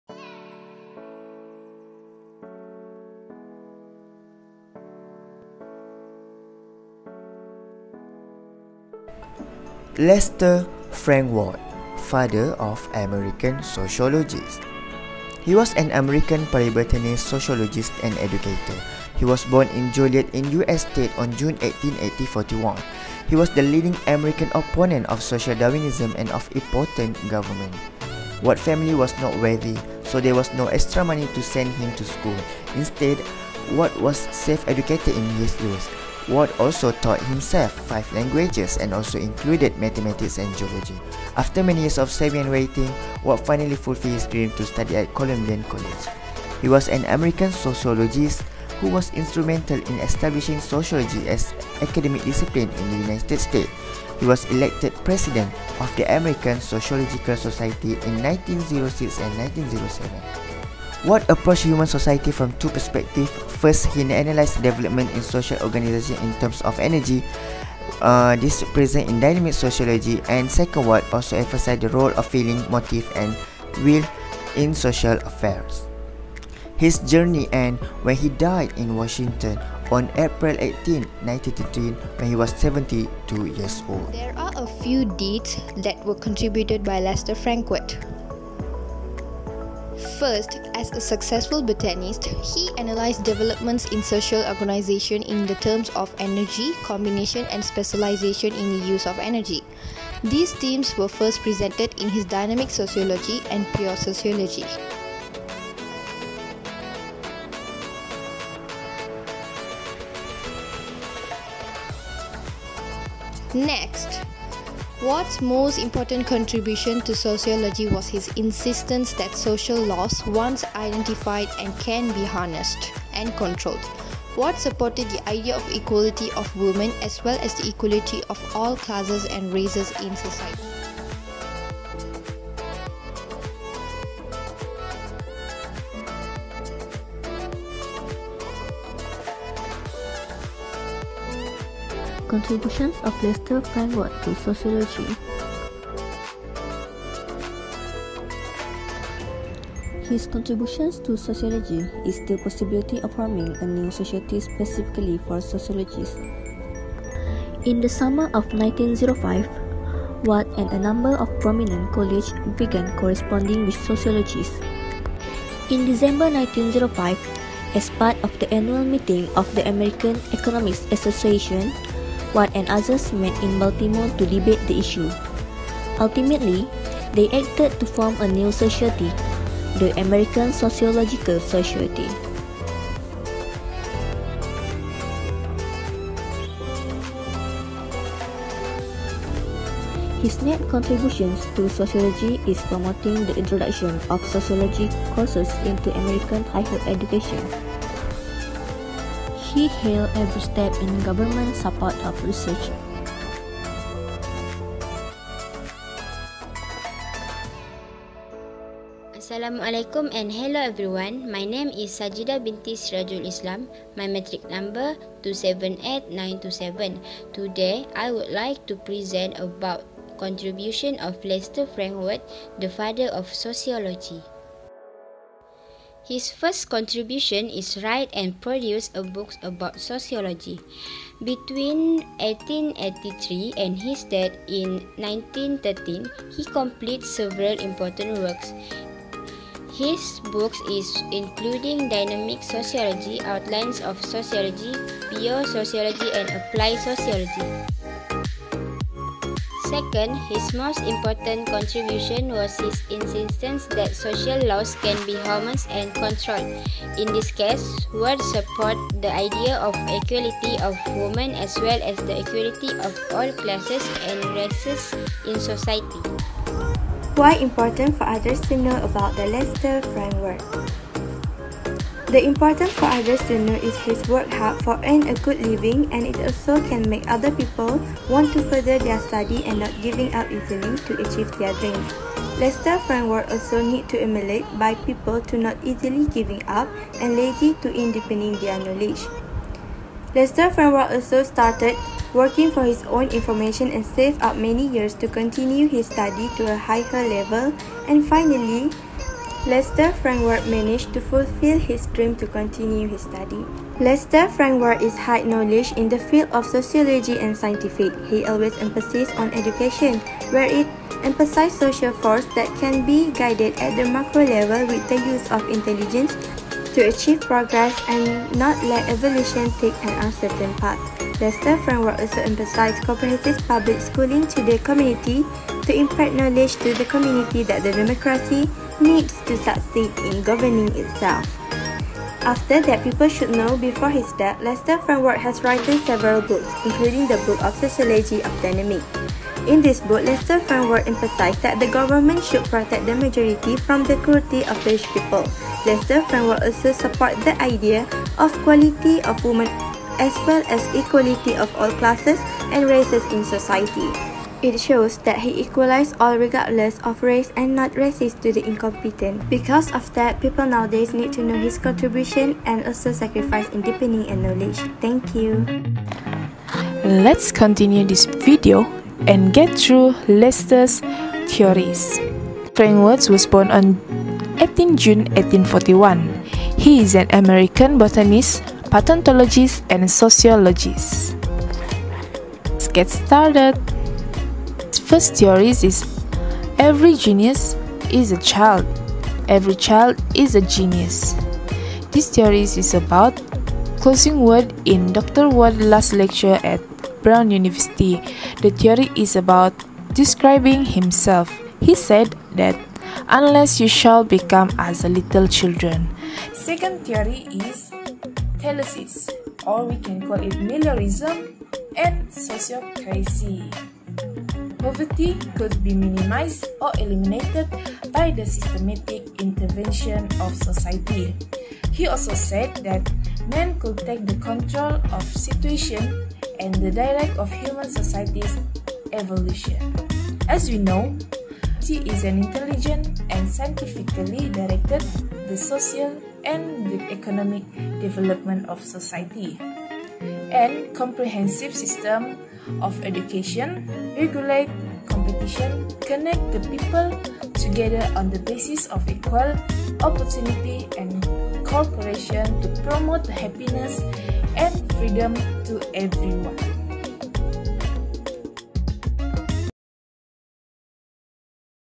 LESTER FRANK WARD Father of Sociology
This video is a Group Assignment for GHZS 2033 Sociology of Tourism. We choose Lester Frank Ward as our figure of Sociologist for this assignment.

This video was prepared by Eva, Danial, Nurin, Sajidah and Dhineshwary.

Hope you guys enjoy it. hiihi